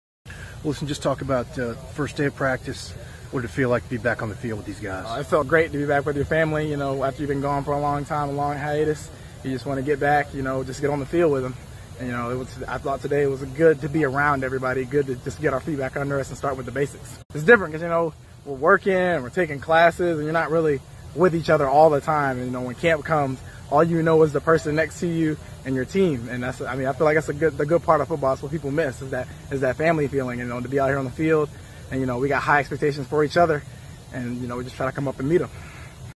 Wofford quarterback Brandon Goodson after first practice of season.
By: Todd Shanesy
Published on: August 1, 2017